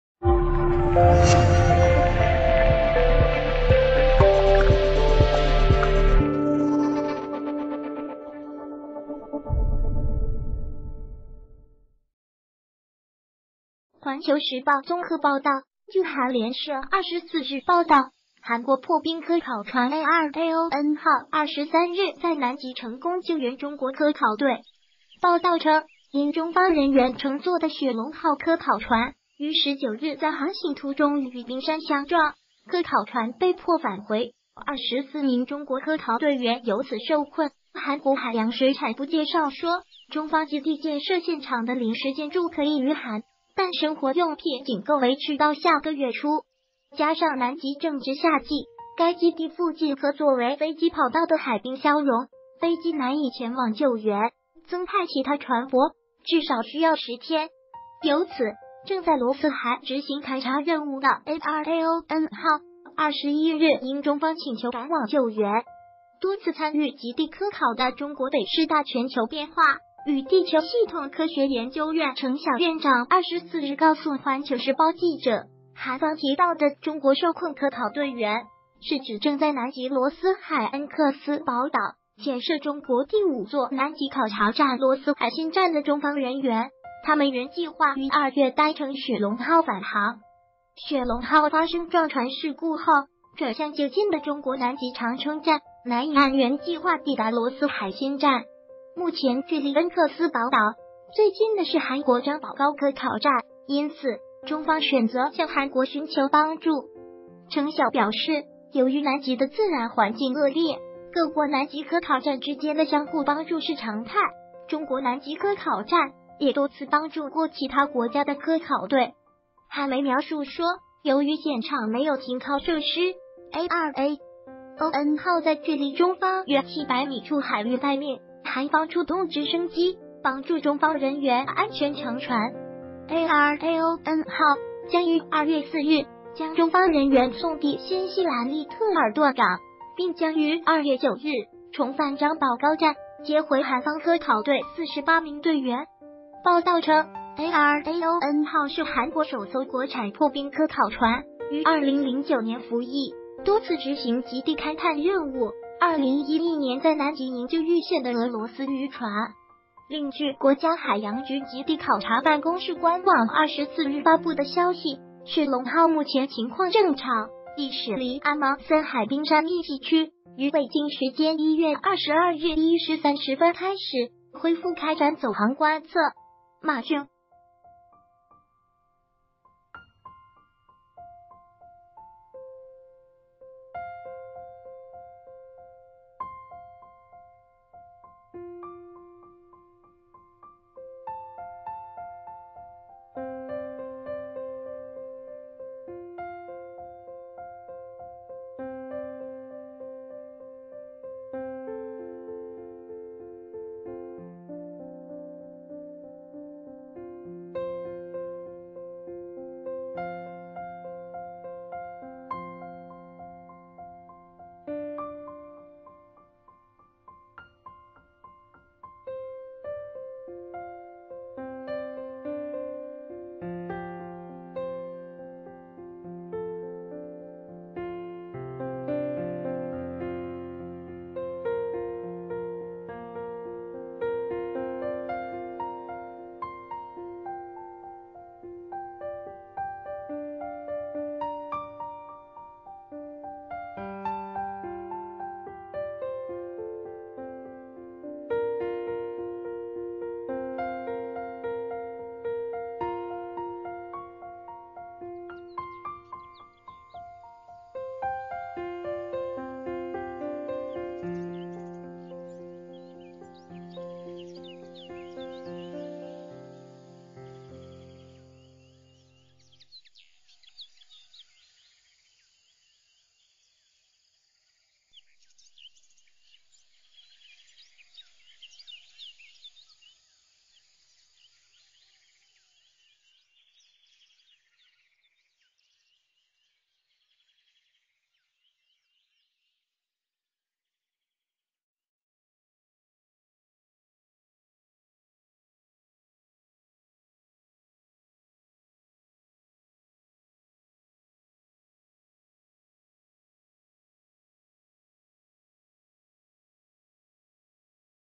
韩媒：韩国破冰船“成功救援中国科考队”
谢谢！【环球时报综合报道】据韩联社24日报道，韩国破冰科考船“ARAON”号23日在南极成功救援中国科考队。报道称，因中方人员乘坐的“雪龙”号科考船于19日在航行途中与冰山相撞，“科考船被迫返回，24名中国科考队员由此受困”。韩国海洋水产部介绍说，中方基地建设现场的临时建筑可以御寒，但生活用品仅够维持到下个月初。加上南极正值夏季，该基地附近可作为飞机跑道的海冰消融，飞机难以前往救援，增派其他船舶至少需要10天。由此，正在罗斯海执行勘察任务的“ARAON”号21日应中方请求赶往救援。多次参与极地科考的中国北师大全球变化与地球系统科学研究院程晓院长24日告诉《环球时报》记者，韩方提到的“中国受困科考队员”是指正在南极罗斯海恩克斯堡岛建设中国第五座南极考察站——罗斯海新站的中方人员，他们原计划于2月搭乘“雪龙”号返航。“雪龙”号发生撞船事故后，转向就近的中国南极长城站，难以按原计划抵达罗斯海新站。目前距离恩克斯堡岛最近的是韩国张保皋科考站，因此中方选择向韩国寻求帮助。程晓表示，由于南极的自然环境恶劣，各国南极科考站之间的相互帮助是常态，中国南极科考站也多次帮助过其他国家的科考队。韩媒描述说，由于现场没有停靠设施，“ARAON”号在距离中方约700米处海域待命，韩方出动直升机帮助中方人员安全乘船。“ARAON”号将于2月4日将中方人员送抵新西兰利特尔顿港，并将于2月9日重返张保皋站接回韩方科考队48名队员。报道称，“ARAON”号是韩国首艘国产破冰科考船，于2009年服役，多次执行极地勘探任务，2011年在南极营救遇险的俄罗斯渔船。另据国家海洋局极地考察办公室官网24日发布的消息，“雪龙”号目前情况正常，已驶离阿蒙森海冰山密集区，于北京时间1月22日1时30分开始恢复开展走航观测。▲（马俊）